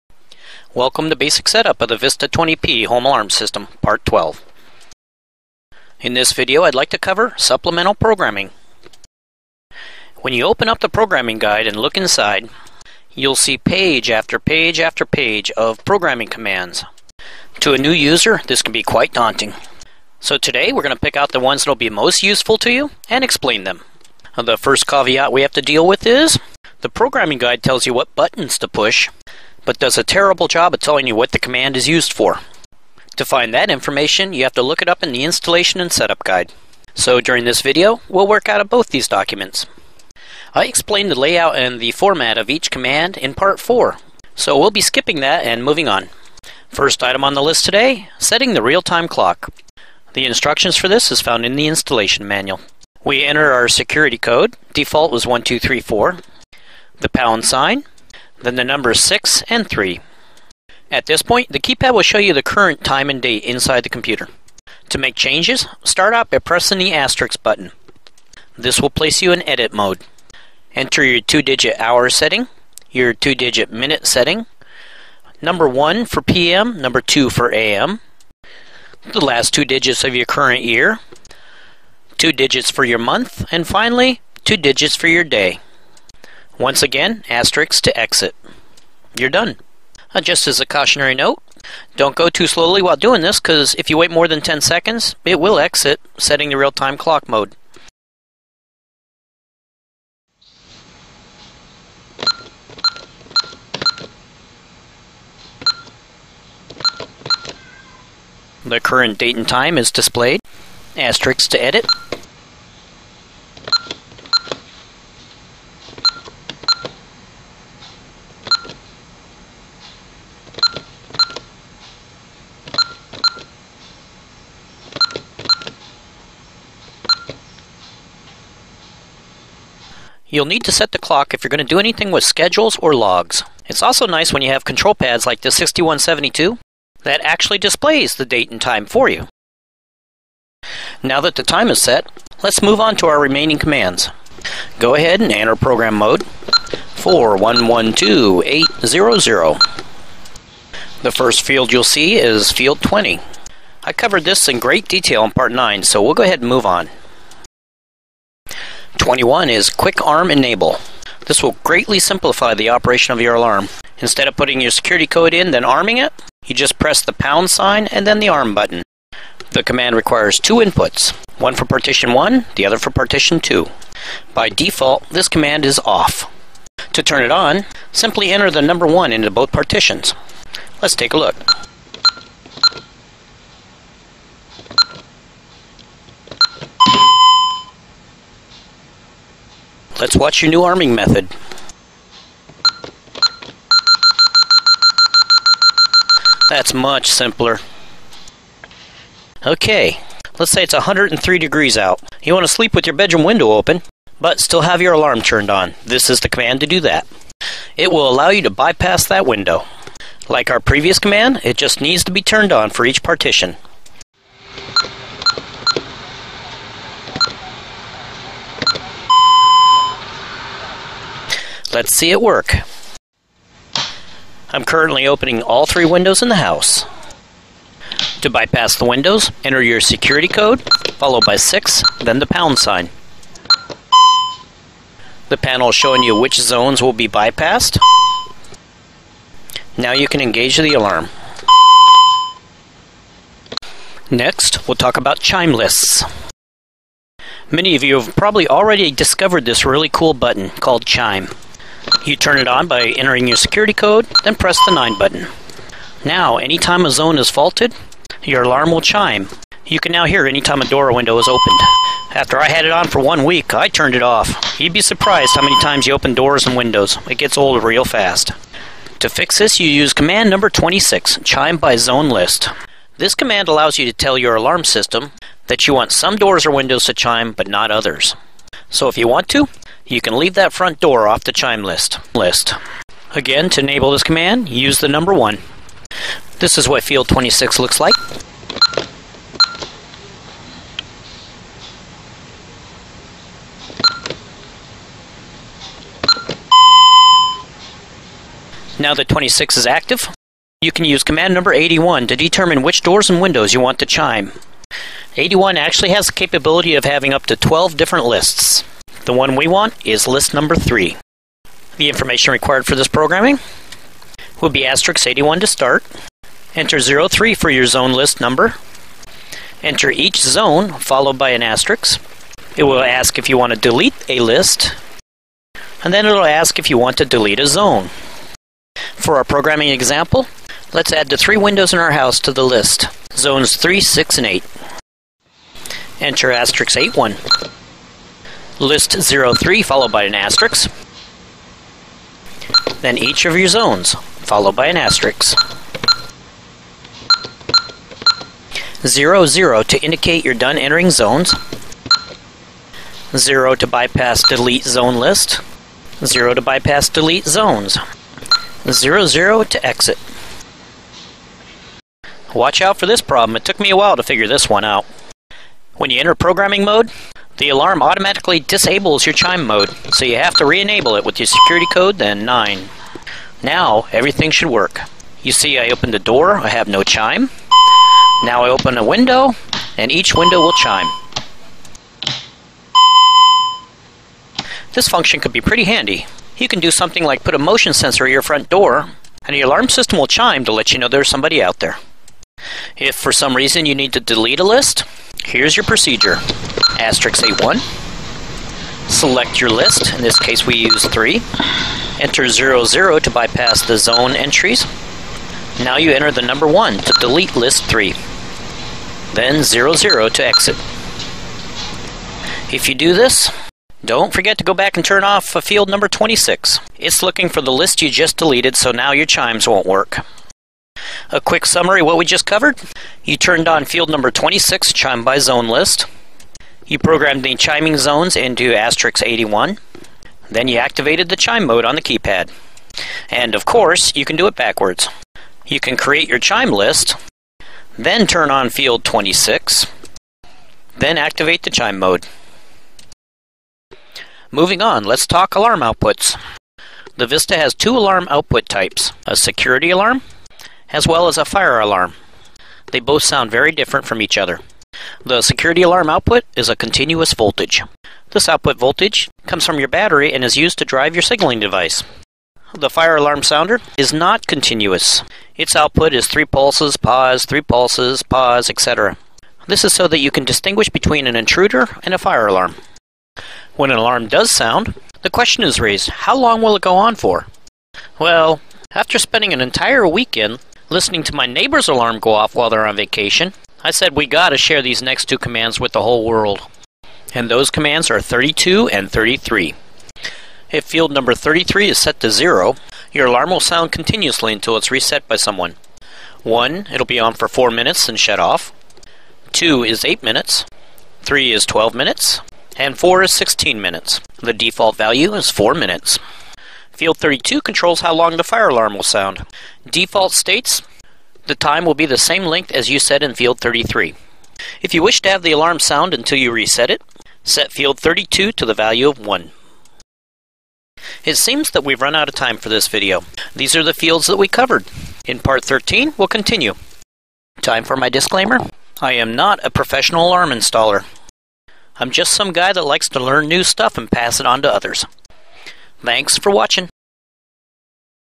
supplemental programming - 1 (Vista 20p part 12)
This video explains in detail how to program fields 21 (Quick arm enable), 23 (Quick forced bypass), 26 (Chime by zone), 32 (Fire alarm sounder output), 33 (Alarm sounder timeout) and Menu command # 81 (Zone lists).
Click on the link below to go directly to the command you are interested in:
0:45 Set real time clock
2:20 Entering programming mode
2:37 (field 21) Quick arm enable
3:18 (field 23) Quick (forced) bypass
4:03 Chime lists
4:27 (field 26) Chime by zone list 3
5:00 (field 81) Zone list programming menu mode
7:24 Alarm output commands
7.35 (field 33) Alarm sounder time out
7:45 (field 32) Fire alarm sounder timeout